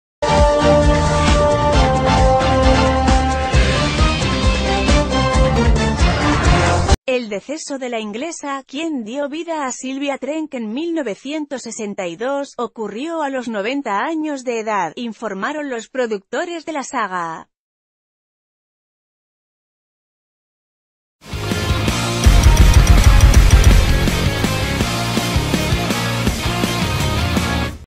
Espectaculos | La Crónica de Hoy
El deceso de la inglesa, quien dio vida a Sylvia Trench en 1962, ocurrió a los 90 años de edad, informaron los productores de la saga.